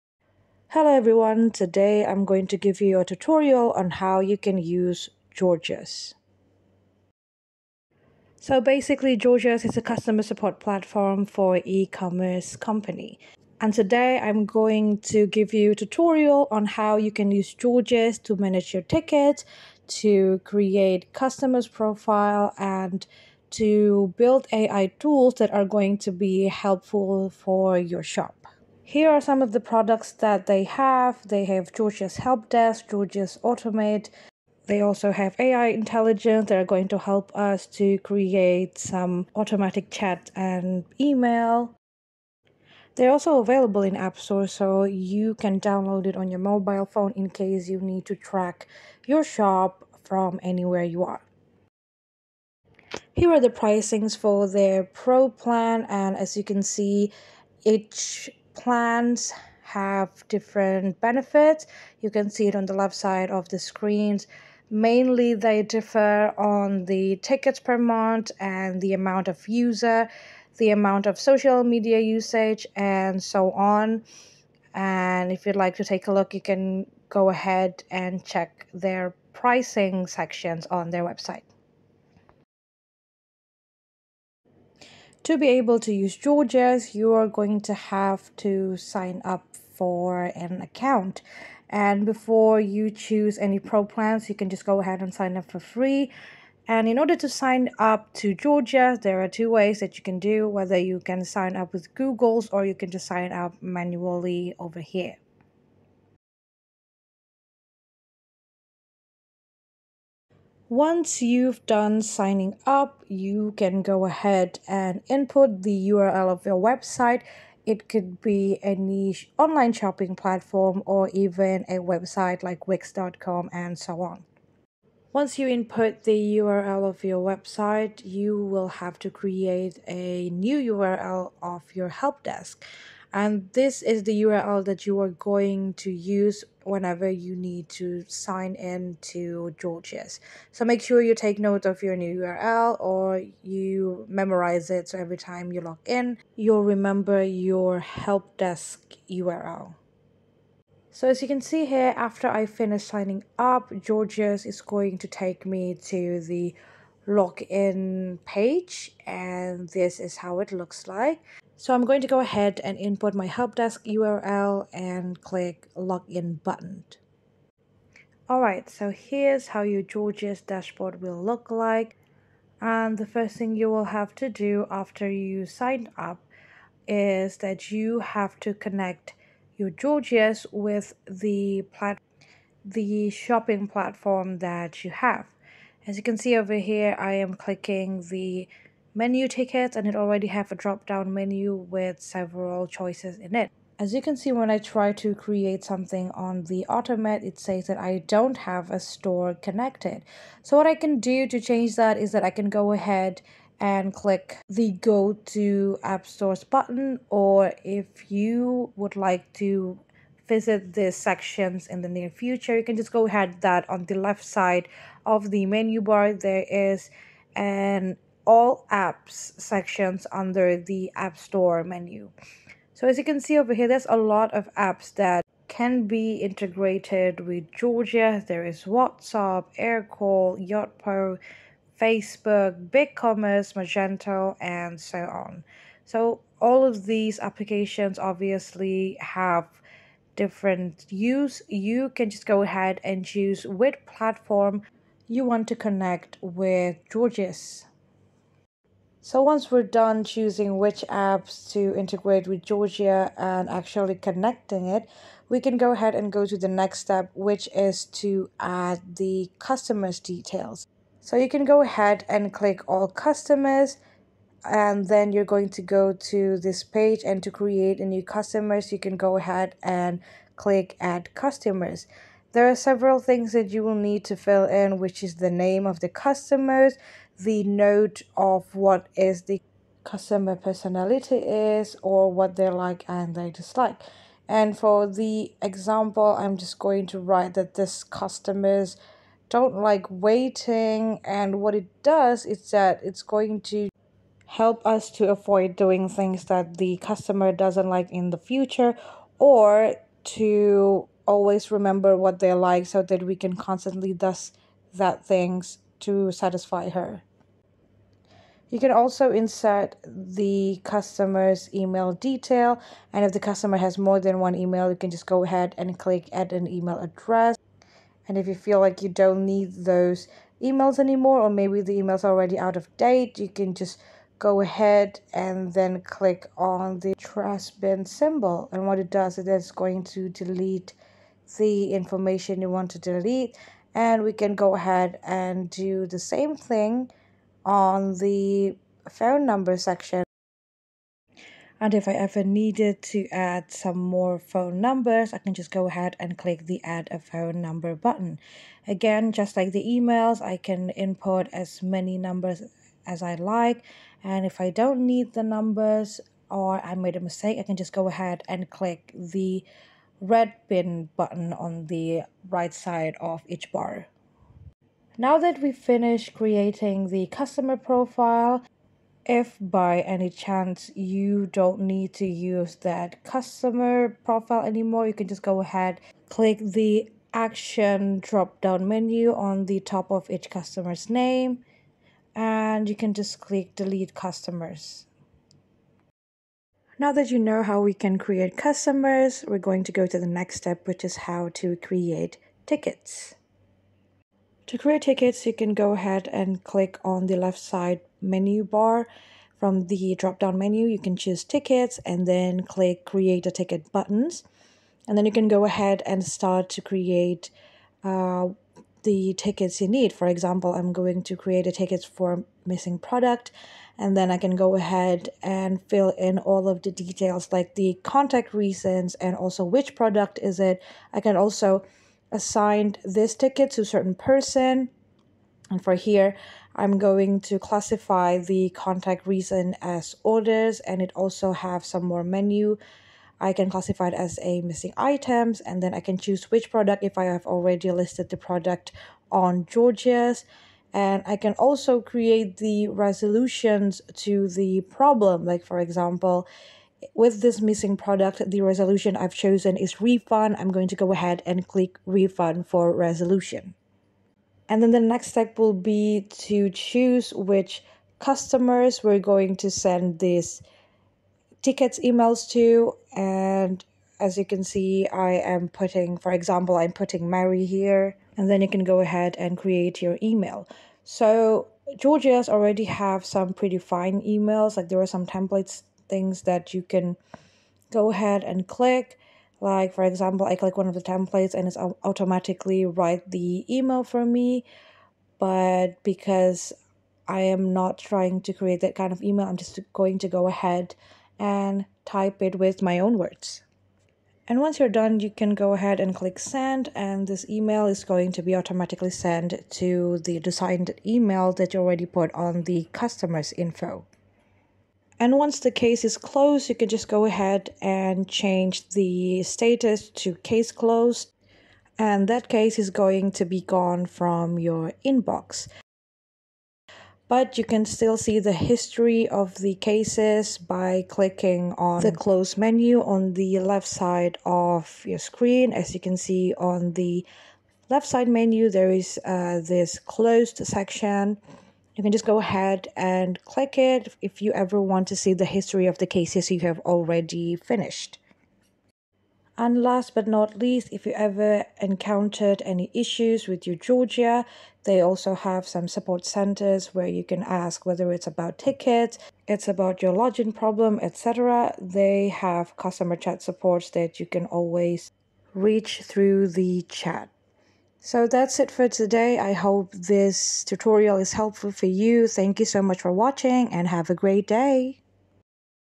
Gorgias Tutorial for Beginners | How to Use Gorgias
If you are looking for a video about Gorgia Tutorial for beginners, here it is!
In this video you will learn how to use gorgias.